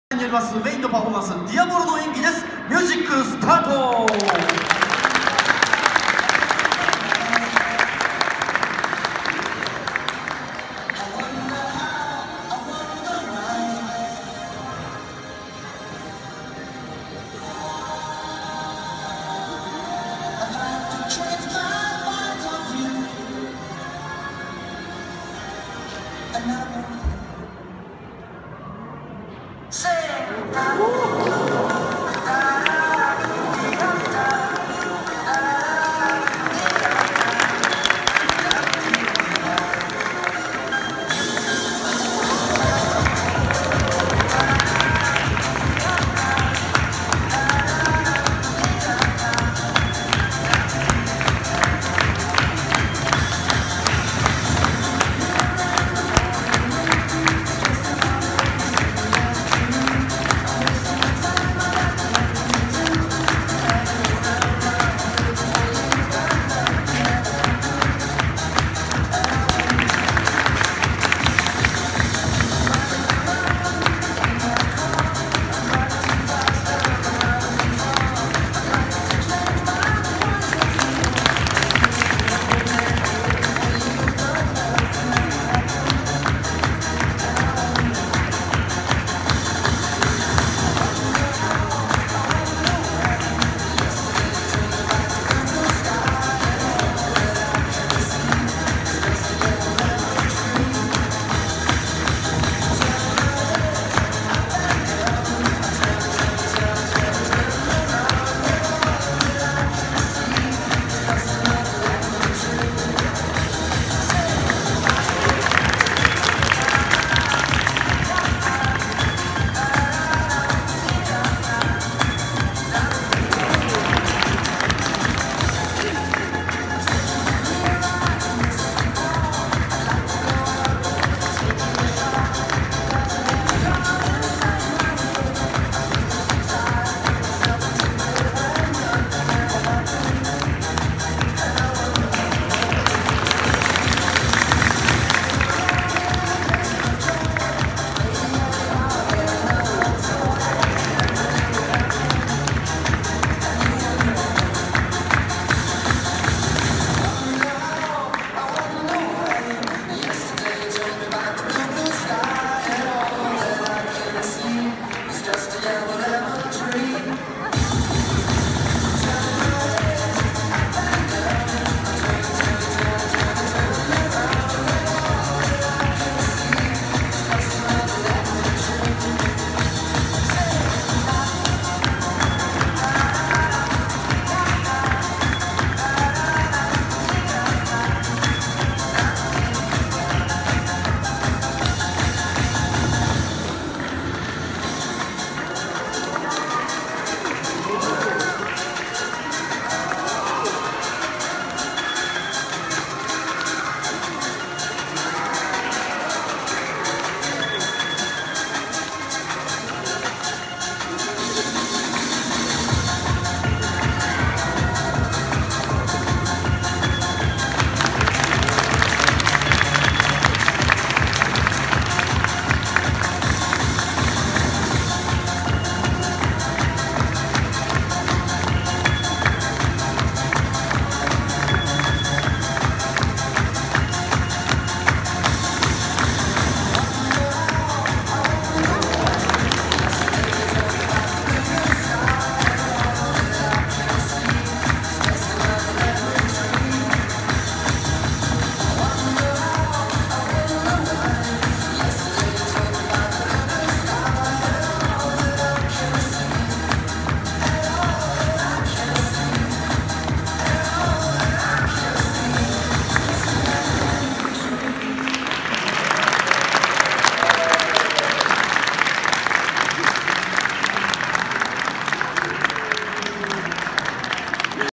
【再掲】ディアボロ スゴ技、圧倒的なジャグリング、ちゃんへん．（CHANG-HAENG.）#1☆うつのみや大道芸フェスティバル
芸歴22年。これまで世界82の国で公演を行ったプロのジャグリングパフォーマーであり、年間100以上も登壇する講演家でもある。
著書『ぼくは挑戦人』。ラスベガス大会2022🥇
MICHAEL JACKSONの前でもパフォーマンスを行った世界的なパフォーマー。圧倒的なテクニックと驚異的なスピードで観る者を魅了するエキセントリックなパフォーマンス。